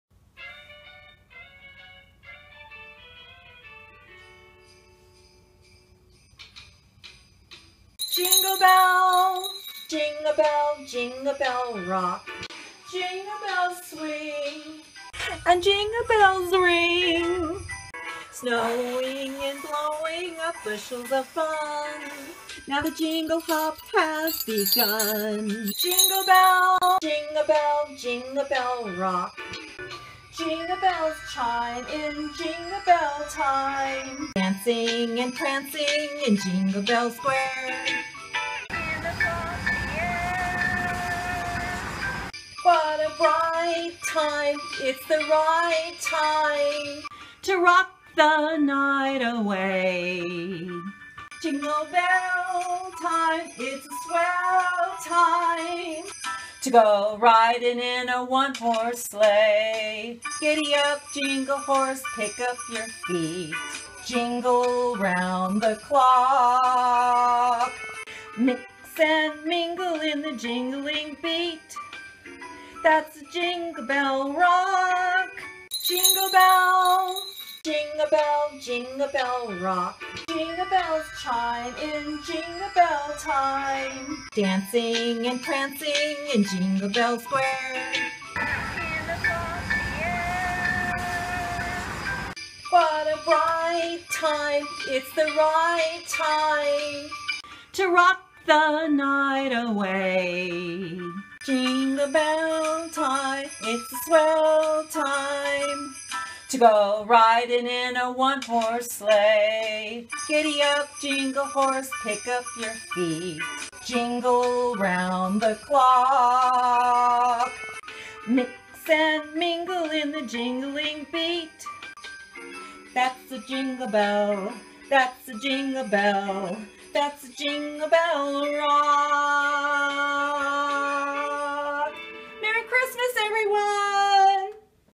Merry Christmas Silly Sing-a-Long with Reborn Baby Dolls!
Merry Christmas everyone!  My singing wasn't "really good" but my heart was in it. I hope you enjoy your gift! Reborns are life-like baby "DOLLS" not real babies.